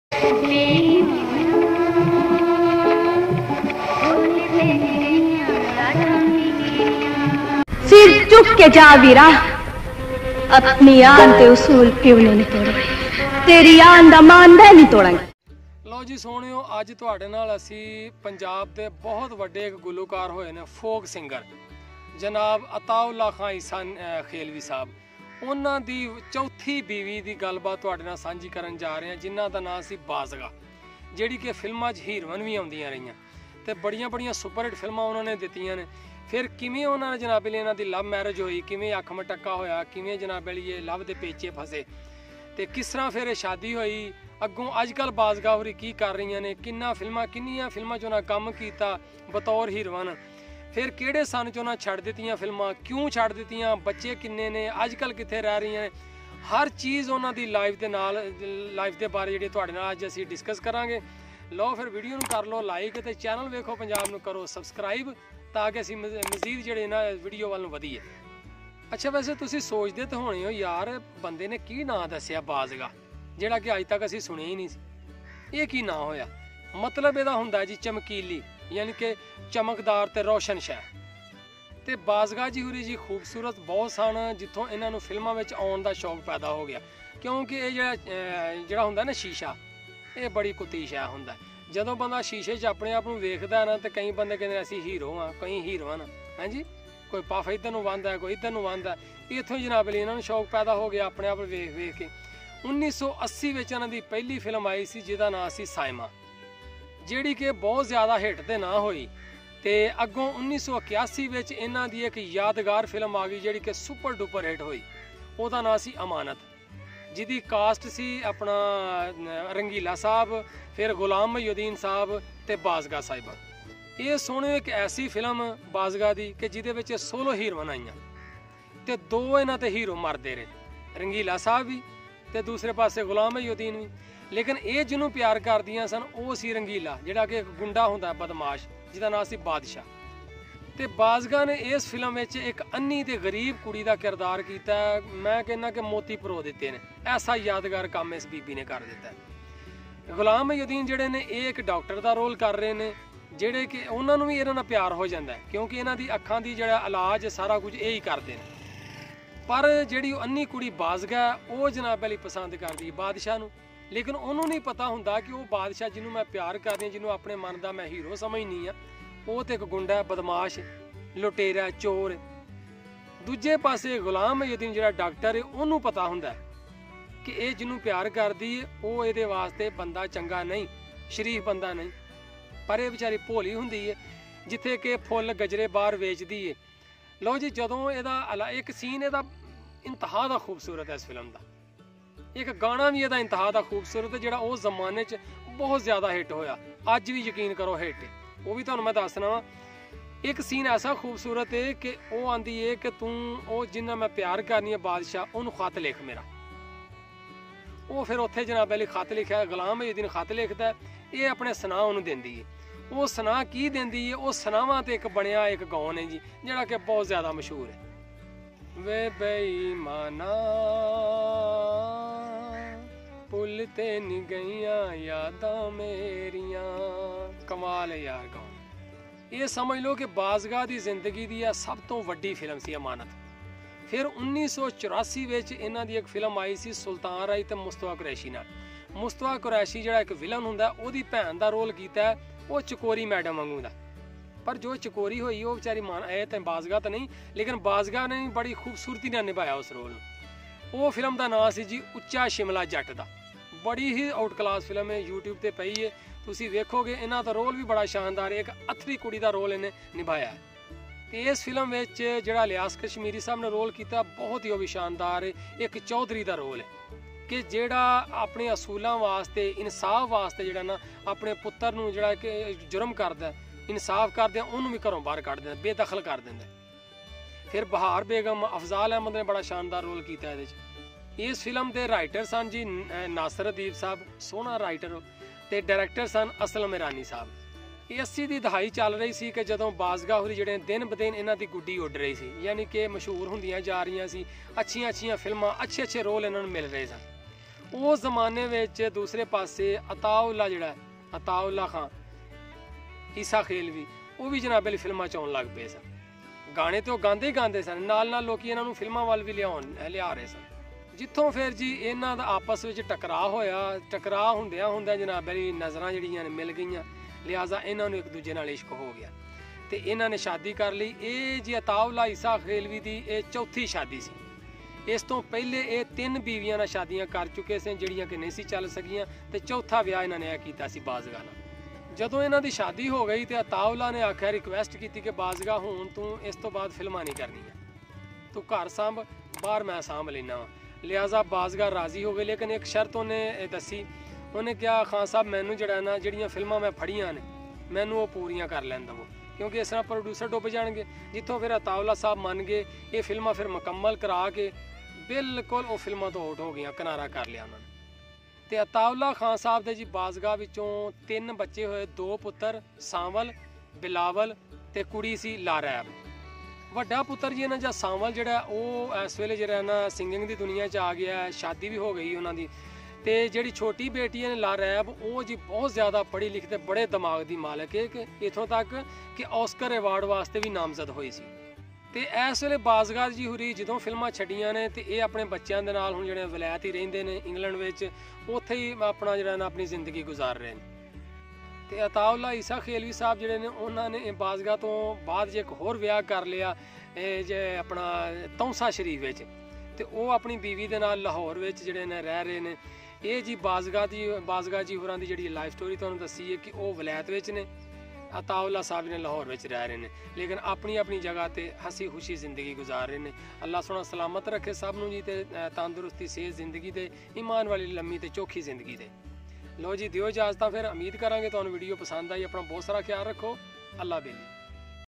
Actress Bazgha Biography | Attaullah Khan Esakhelvi Wife Bazgha Untold Life Story | Sanwal & Laraib
ye video ek biography aur Filmography par manning hai jo hum Pakistani Punjabi Folk Singer Attaullah Khan Esakhelvi ki bivi wife filmstar Bazgha per bana rahe hein jo first film Saima mein actress bani aur phir best movie of bazgha Amanat film thi jis me Actor Rangeela and Ghulam Mohayudin hero thay aur Bazgha heroin thi film ki phir Fantastic Punjabi movie Ucha Shimla Jatt Da jis me Hero Sultan Rahi aur villain Mustafa Qureshi thay jabky film me side heroin ka role Attaullah Esakhelvi ki biwi Bazgha ne kia tha jo Chakori ki tarah ka best role tha. aajkal Bazgha apne sons Sanwal Esakhelvi and Bilawal Esakhelvi and Laraib Esakhelvi ke sath England me rehti hain aur Attaullah Khan Esakhelvi apni panchvi wife ke sath Lahore me rehty hain. ye kahani hai biography hai film actress Bazgha ki so must watch and subscribe channel Wekho Punjab . Thanks
Sajjad Ahmed Dadra Jatt